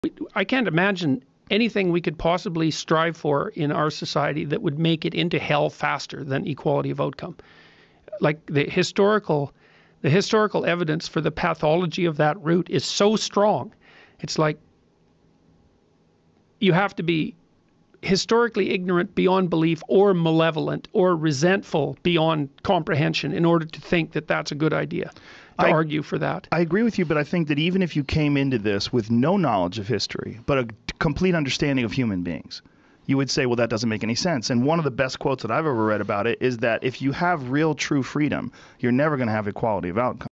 Joe Rogan & Jordan Peterson- Equality of outcome is opposite of freedom (41 sec)
Curated by “Steve’s CLIPS” 2021.